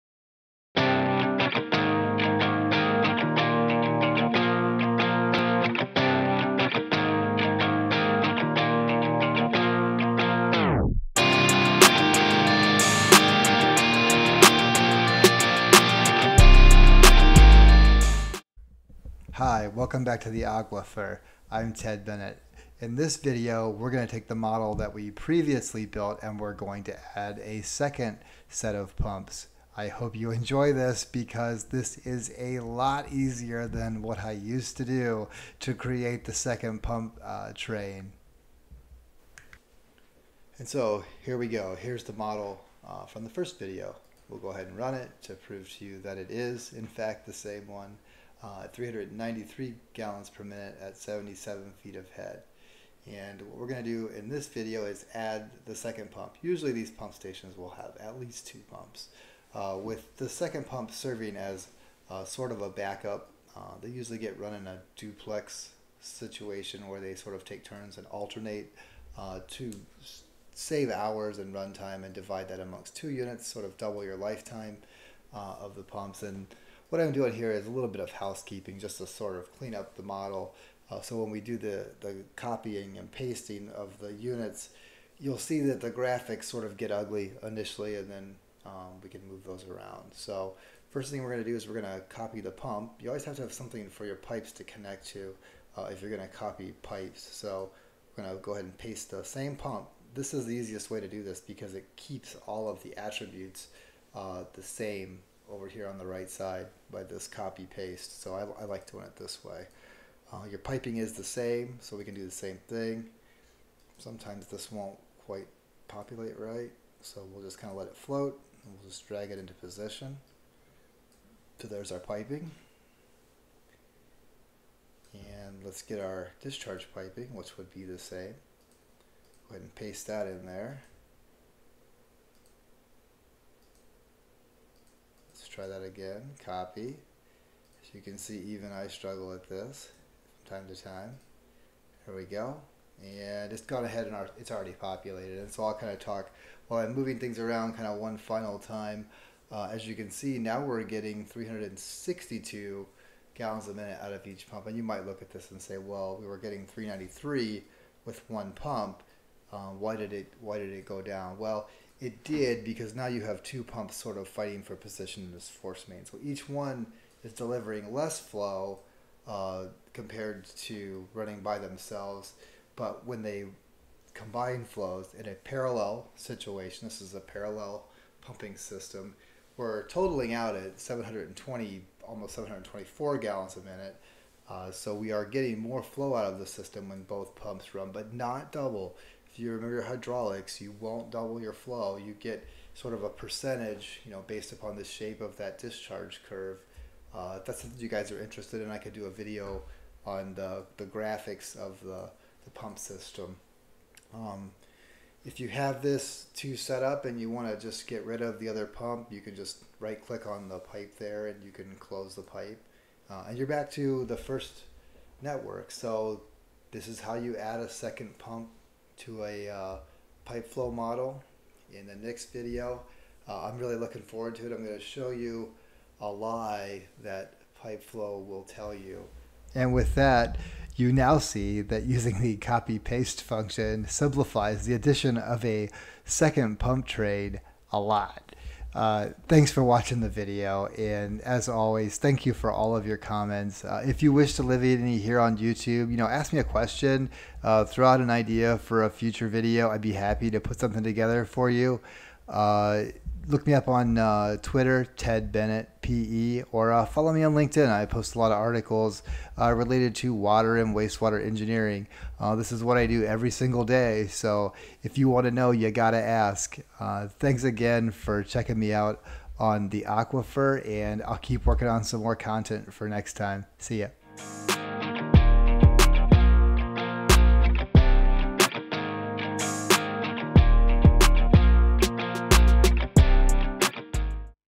Modeling a 2 Pump Sanitary Sewer System Using PIPE FLO
In this video we will take the model that we developed in the first video and easily add a second set of pumps.  The second set of pumps will represent the typical duplex - lead / lag arrangement found in most sanitary sewer submersible pump stations.